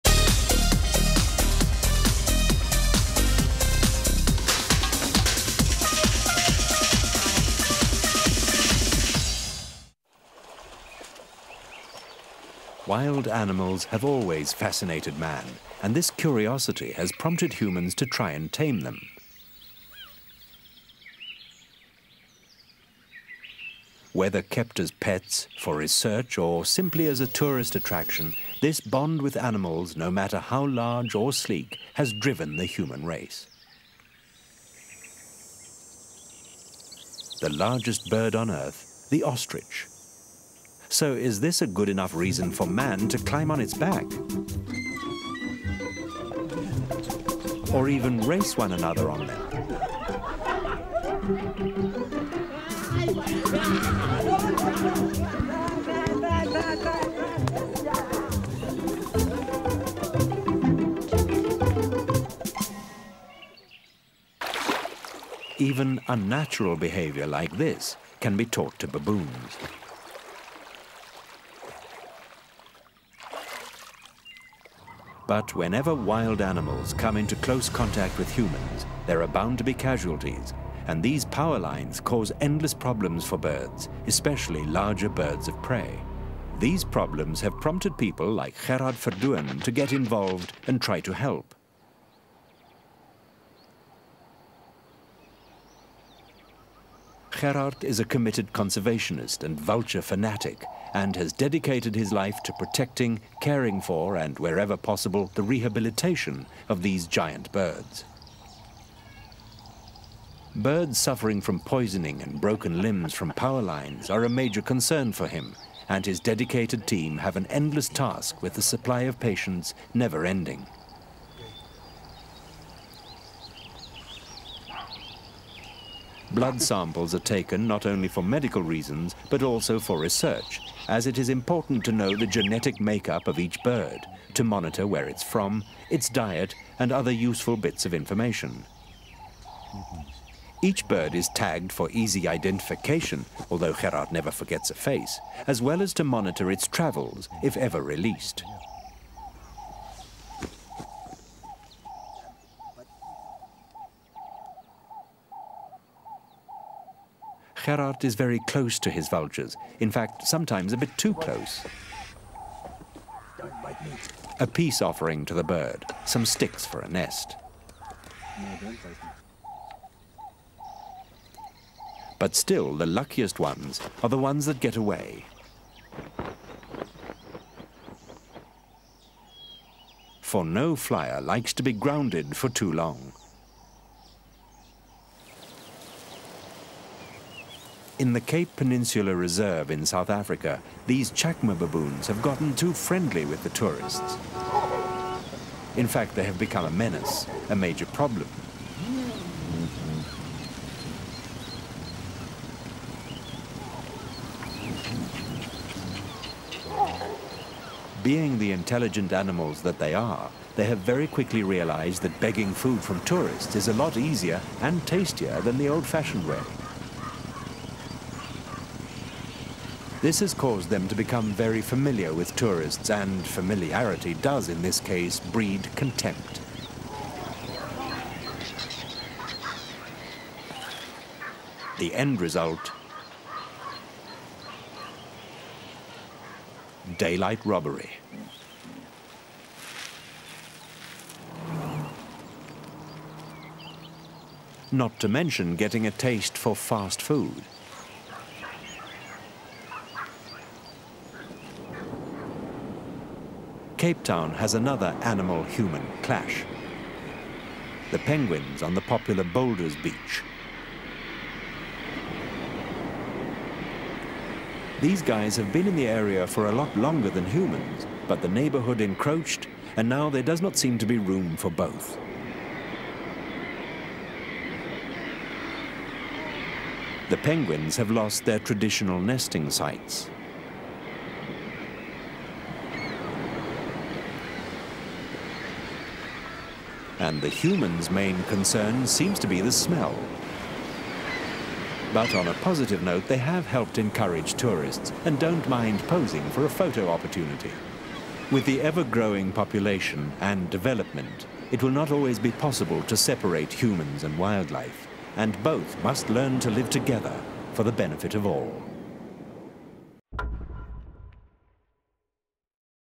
Urban Terror – Urban Animals - Animal Antics - Go Wild
Uncontrolled urbanization is a problem which besets most developing countries and mankind has always tried to conquer and tame wild animals which has inevitably caused problems. This insert highlights the topic.